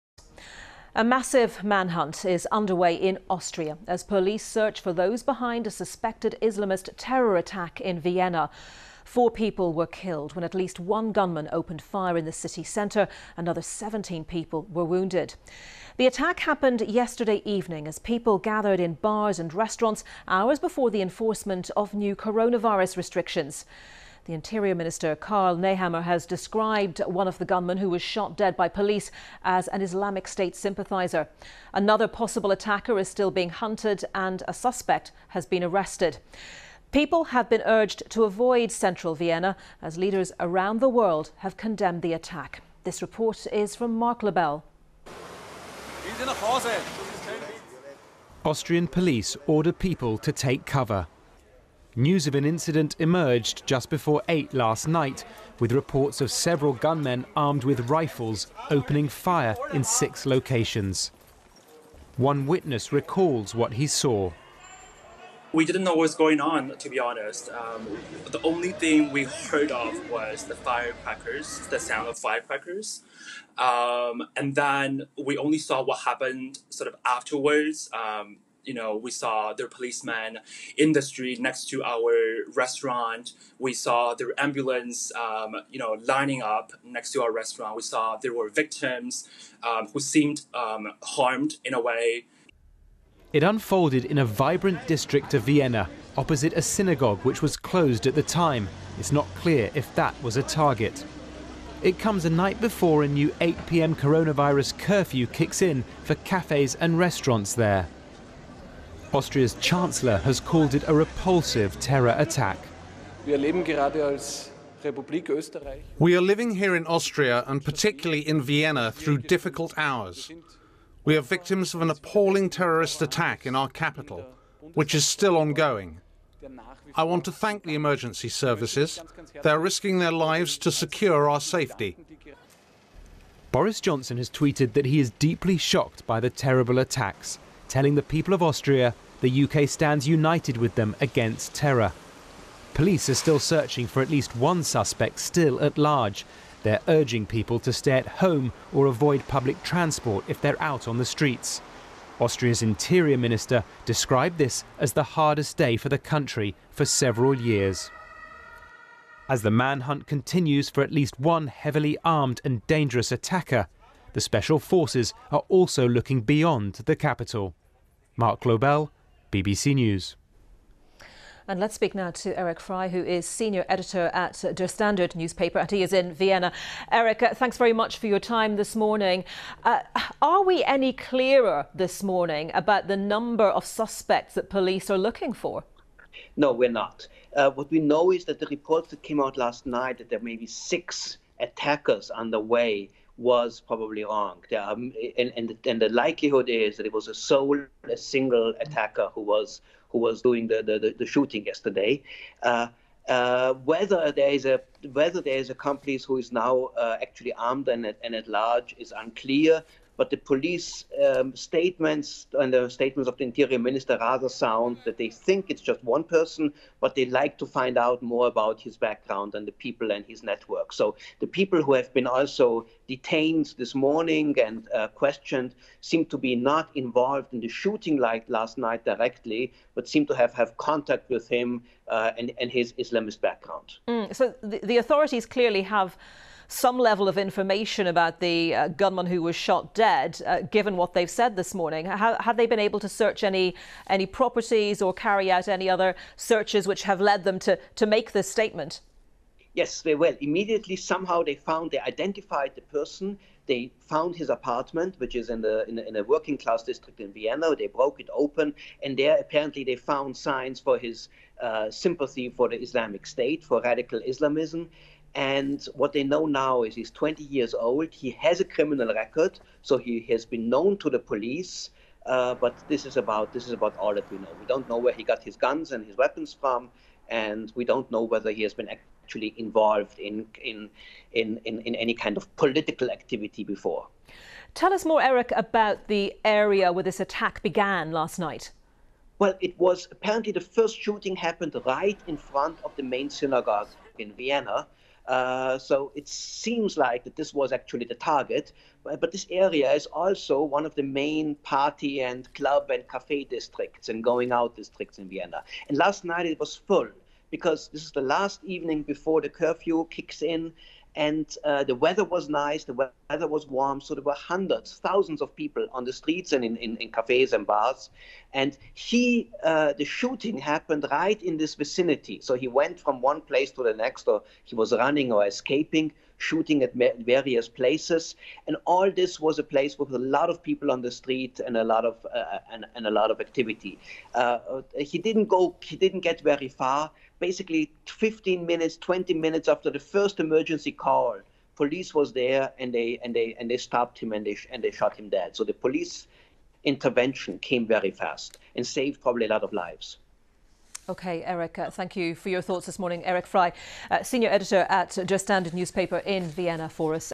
Vienna shooting: Austria hunts suspects after 'Islamist terror' attack - BBC News
Austrian police are searching for at least one suspect after a multiple gun attack in the capital, Vienna, that killed four people.

Seventeen other people have been wounded - some seriously - after gunmen opened fire at six different locations in the city centre on Monday evening.

One attacker was shot dead by police, officials said.

Interior Minister Karl Nehammer described the assailant killed by police as an "Islamist terrorist".

Two of those who died in the random shooting were women and two were men. One of the women was reportedly a waitress. The second woman died of her wounds in hospital overnight, reports said.

The victims were in a city centre area busy with people in bars and restaurants, near Vienna's central synagogue, but it is not yet clear if that was the target.